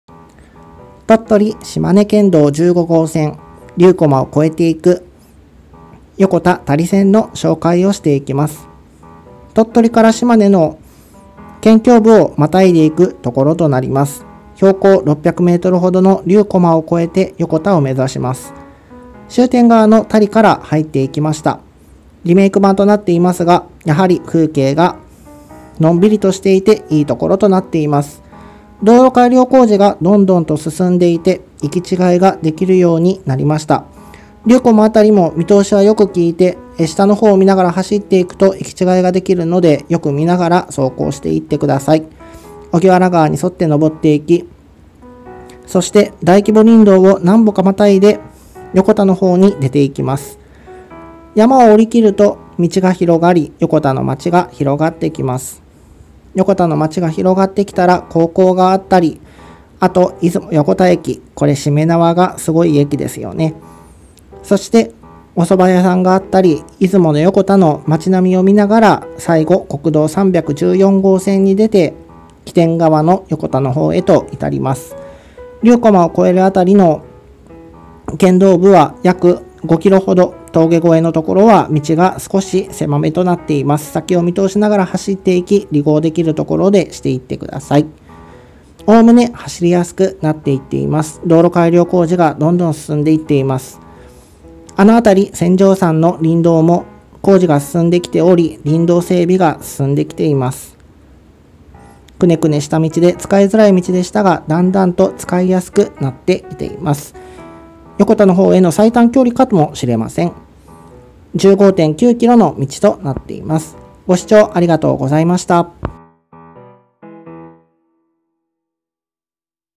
鳥取島根県道15号線横田多里線15.9km竜駒道路改良進む山越え（re走行）
鳥取島根県道15号線横田多里線
起点島根県奥出雲町横田（R314）
終点鳥取県日南町多里（R183）
総延長15.9km
走りやすさ
A 多里～荻原
D 荻原～竜駒～横田
A 横田

鳥取島根県道15号線リメイク版。多里からスタート荻原にかけて遠くに山をみながら川に沿い集落を走る。船上山林道を越えると道が狭くはなりますが前ほどの圧迫感はなくなっています。竜駒を越えてからも道路改良が進み見通しがきいて離合箇所も整備されている。山をおりきり道が広がると横田に下りる。木次線出雲横田駅を越えると起点に至る。